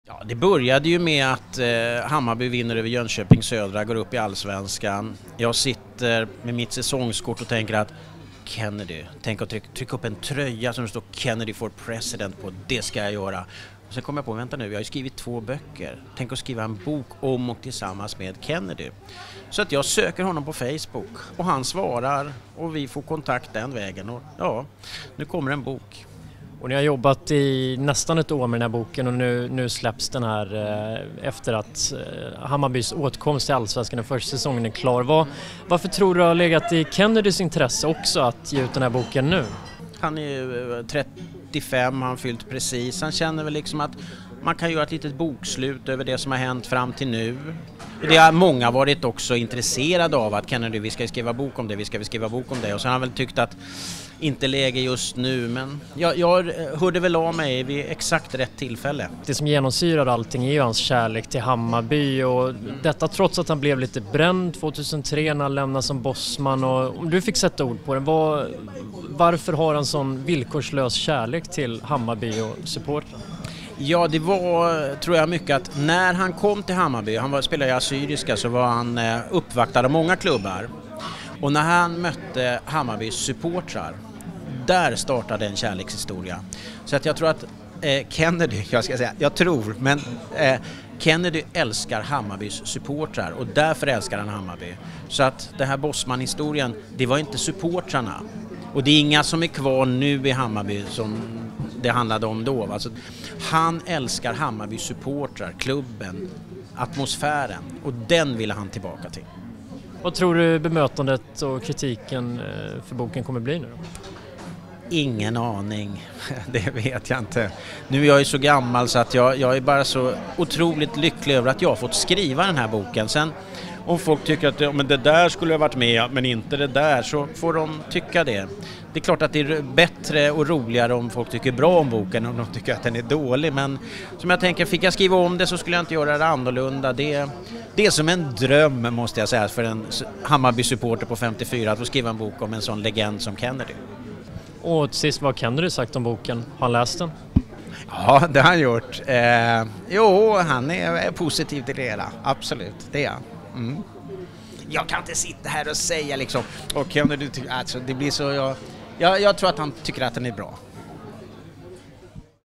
"Kennedy älskar Hammarbys supportrar"
I helgen släpps "Säg oh, ah, Kennedy" - Hammarbys Kennedy Bakircioglus självbiografi. Expressen träffade medförfattaren Benny Haag för att prata om skrivprocessen, samarbetet och idolen Kennedy.